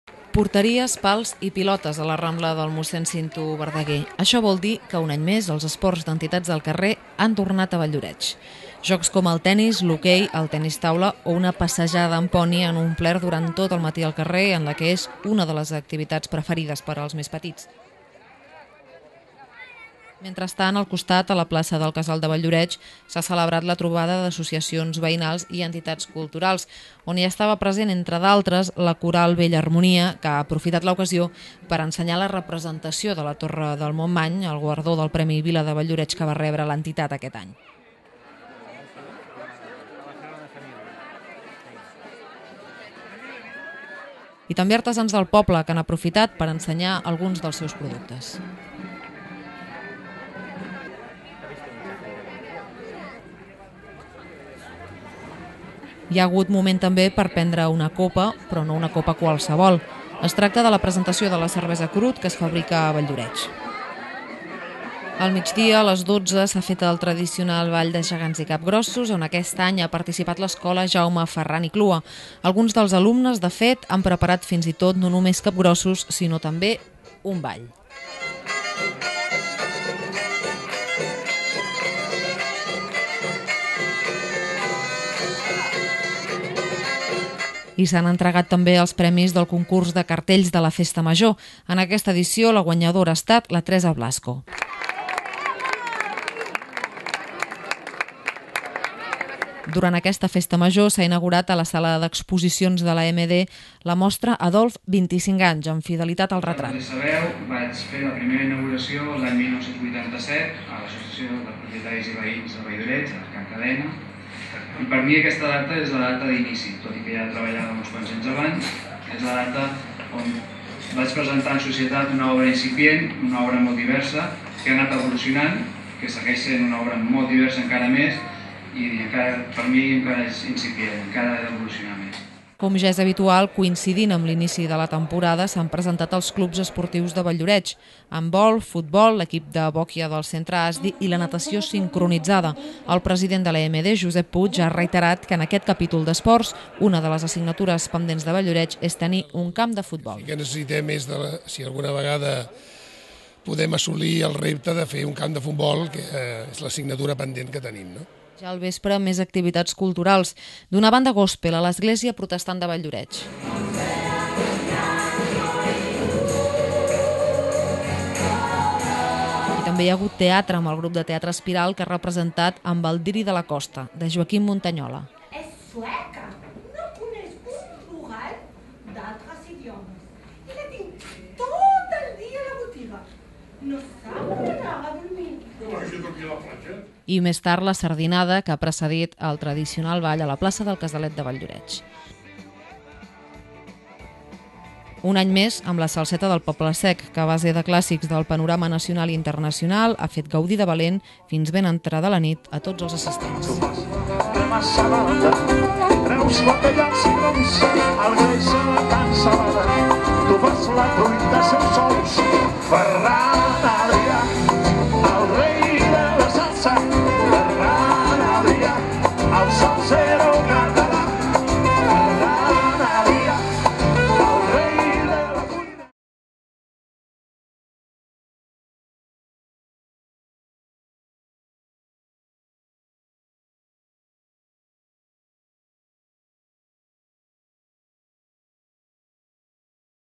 Dissabte de Festa Major a Valldoreix
Esports al carrer, fira d'artesans, mostra d'entitats del poble, ball de gegants teatre i la salseta del poble sec, són alguns dels actes d'aquest dissabte de Festa Major de Valldoreix